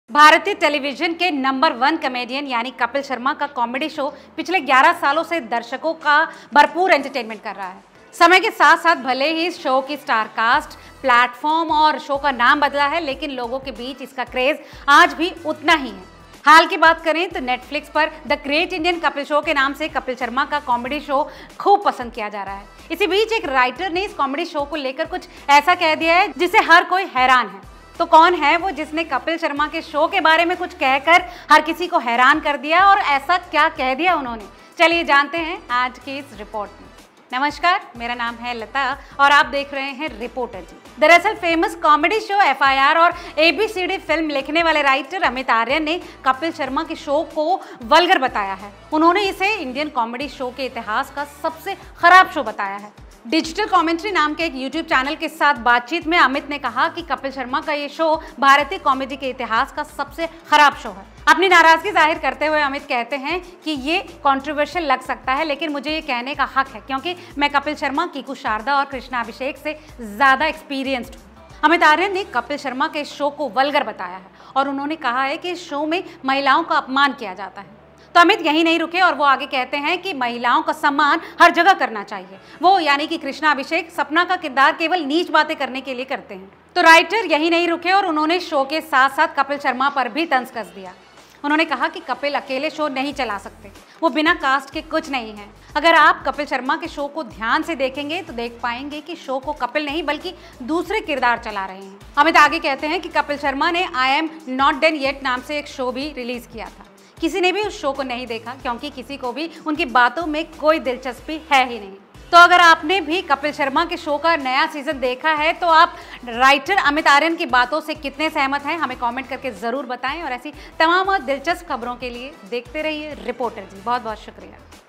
Bollywood Special : Film Writer ने बताया Kapil Sharma Show को सबसे घटिया।
Film Writer ने बताया Kapil Sharma Show को सबसे घटिया।